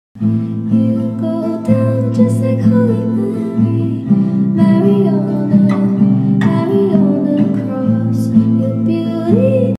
Anyway The Wind Blows - Pride Pick!
Another pick we have for PrideMonth !
Explore Simon, Baz, and Penny’s lives as they traverse a world of magic, friendship, and their own passionate wishes. This book celebrates love in all of its forms, telling us to embrace our genuine selves and follow the wind wherever it may take us.

Here’s our second pick for the month:

Any way the Wind Blows – Rainbow Rowell

1.Readers can’t help but become invested in Rowell’s vibrant worlds and characters.
2.Rowell’s unique storytelling style seamlessly combines the magical elements of the world of mages with relatable human experiences.
3.Her sharp observations and one-liners lighten the story without losing its emotional weight.
4.Character Development: Penelope learns to stand up for herself, Baz learns to be friendlier, Agatha learns to trust her gut, and Simon learns to accept himself for who he is.